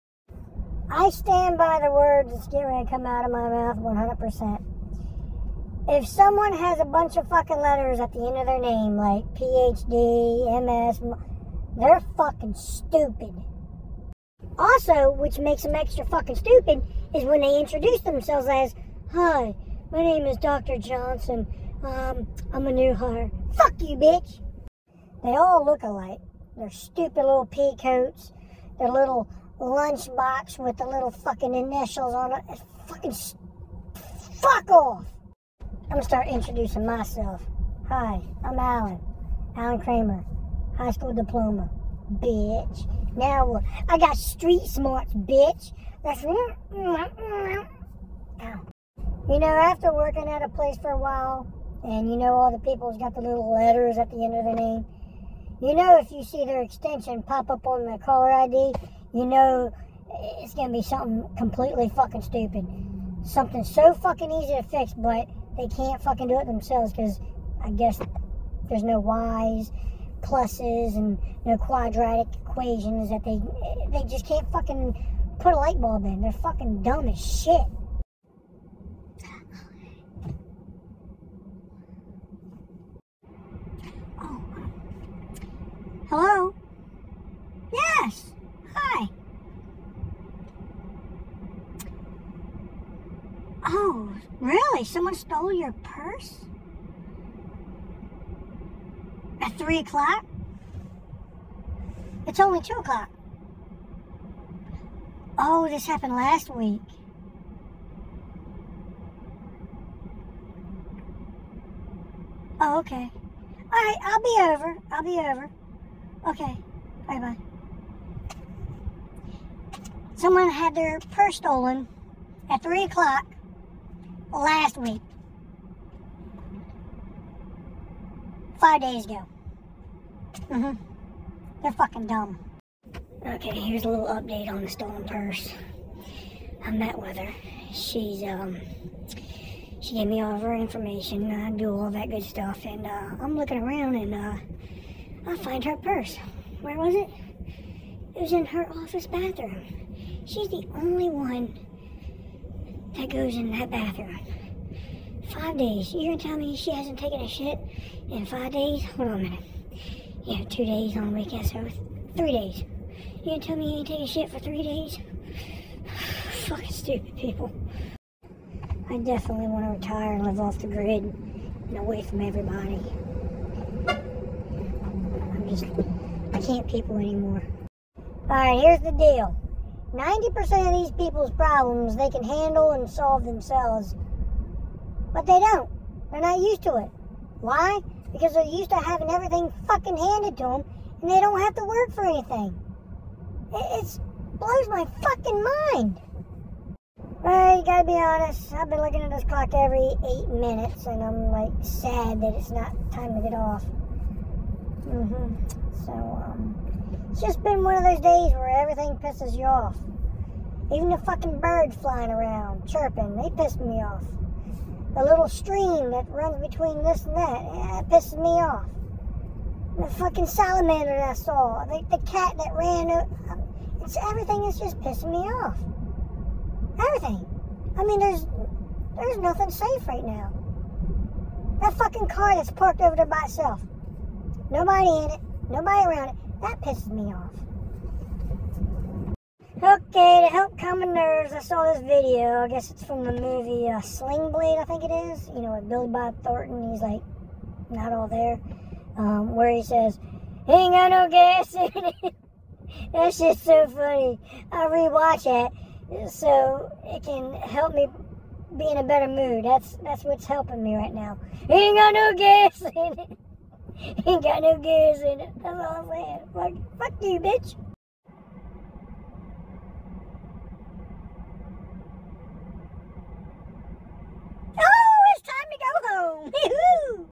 Grumpy
this video of a bunch of short clips spliced together is for entertainment purposes only. no one was injured or harmed, and I absolutely love my job and I work with a bunch of good people. these were done on my breaks and lunch on multiple days. I do have a degree and working on another. so I will have some letters behind my name🤣.